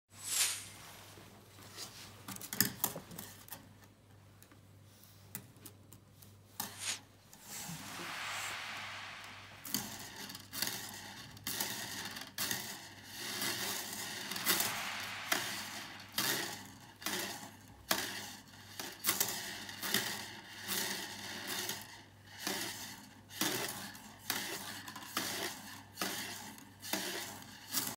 マジックスプリング：地震波説明モデル
ダイソーのおもちゃで、地震の波-P波とS波の違いを視覚的に説明します。詳細
◇都留文科大学　山田暢司